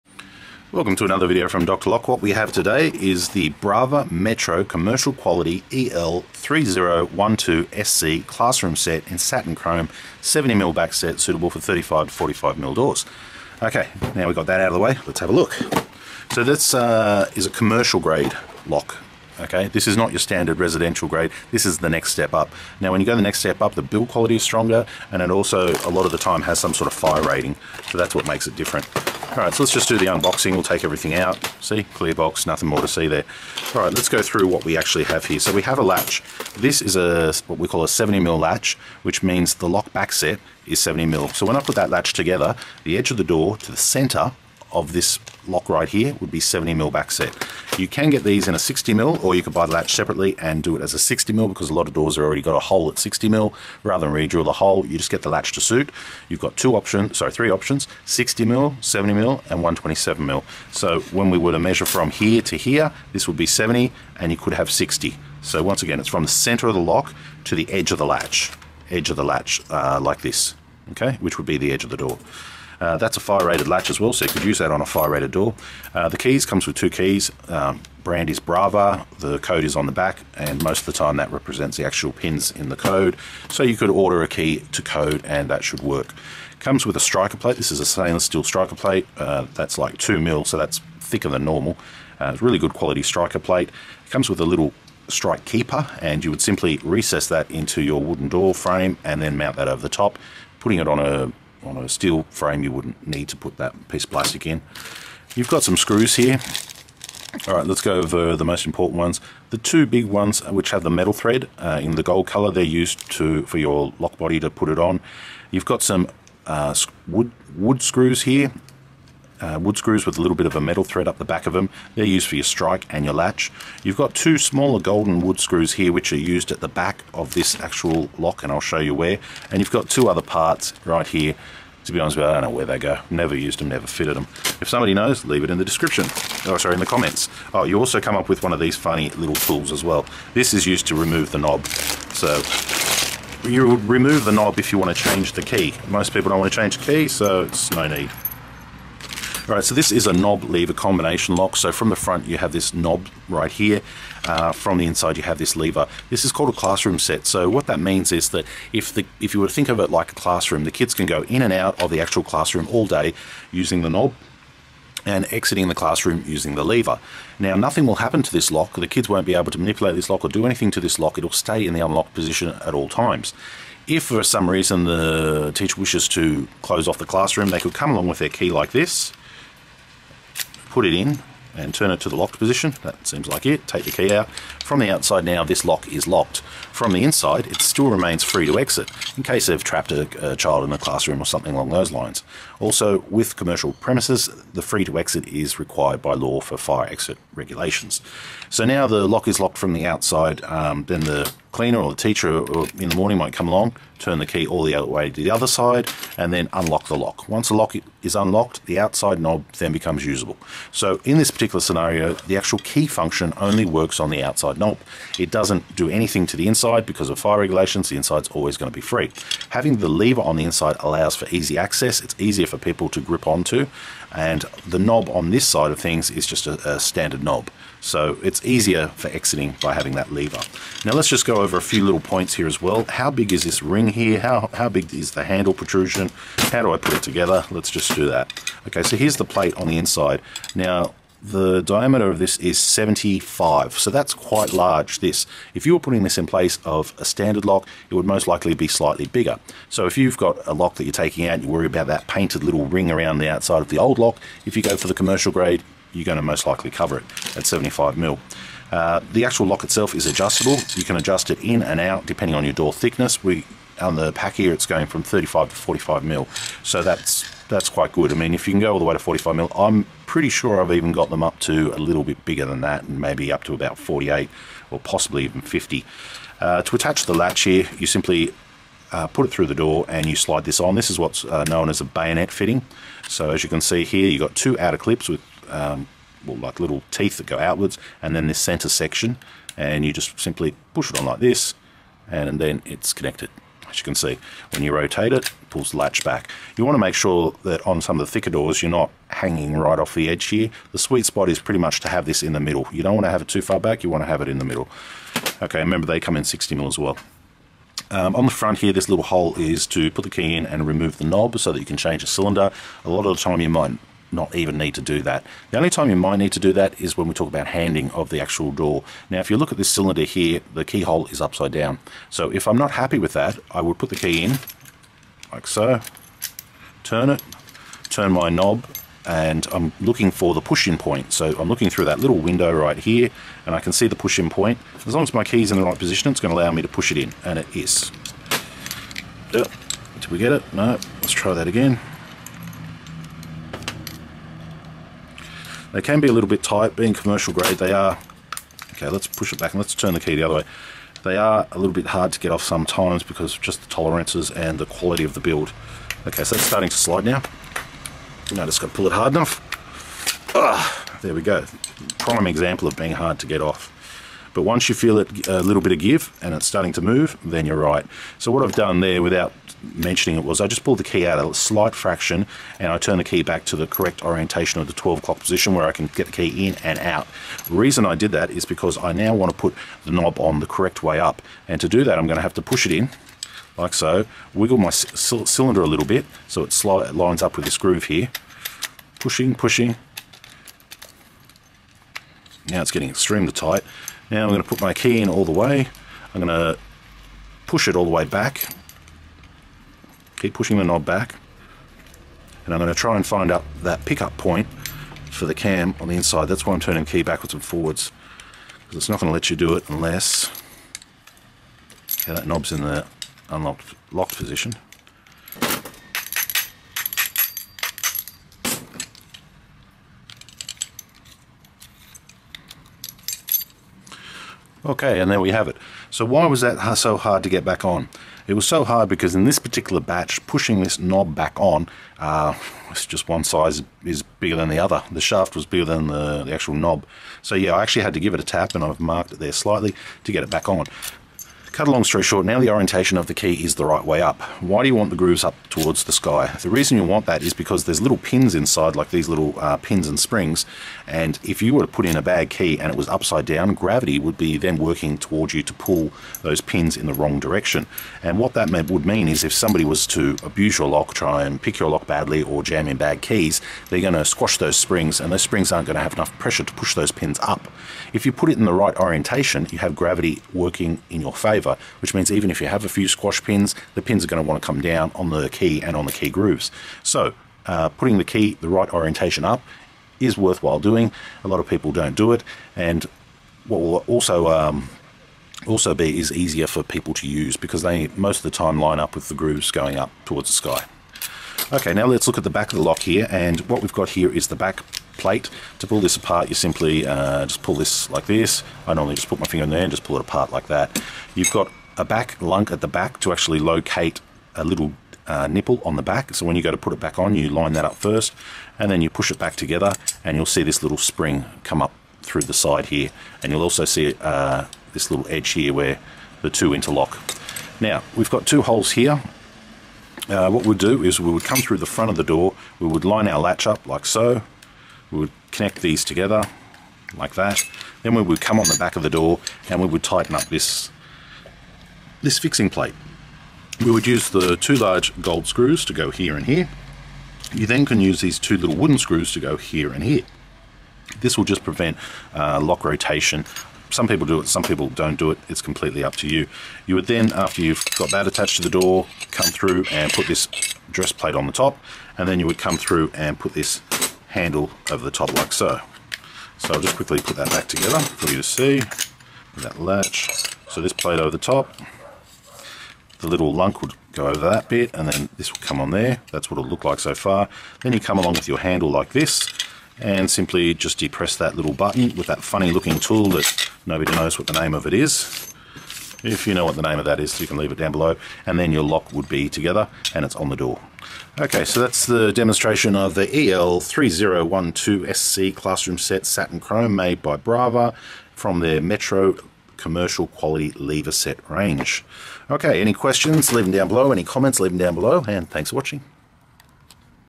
BRAVA METRO CLASSROOM KNOB/LEVER 3012 70MM SC EA3012SS70
BRAVA METRO CLASSROOM KNOB/LEVER 3012 70MM SC. This video will show the Brava EL3012 classroom function know lever combo lock and how to set up the lock and also some tips on installing the lock. See the link below to purchase this lock.Part No. EA3012SS70